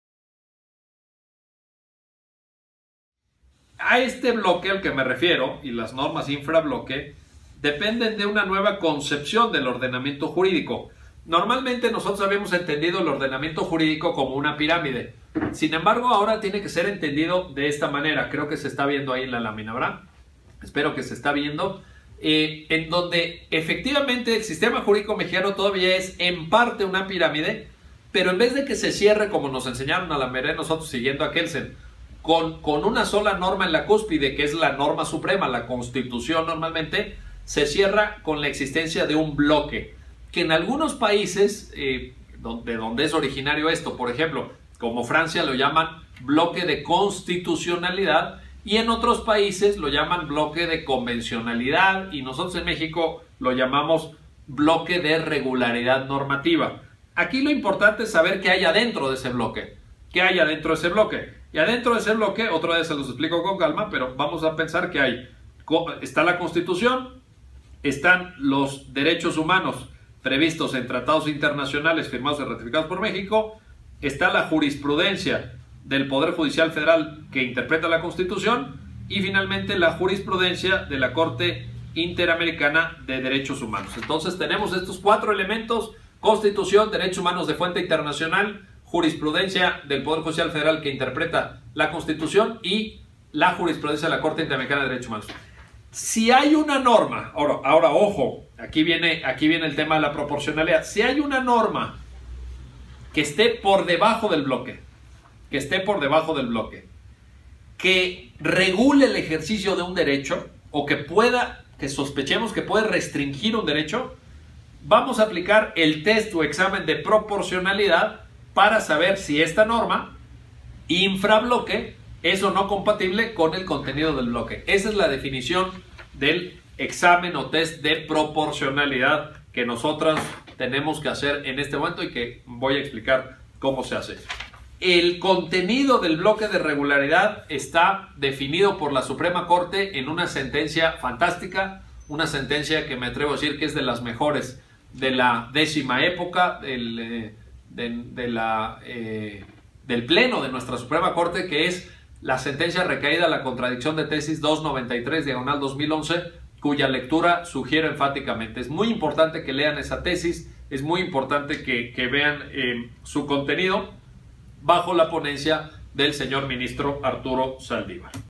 ¿Qué es el bloque de regularidad normativa?
Miguel Carbonell explica en qué consiste el llamado bloque de regularidad normativa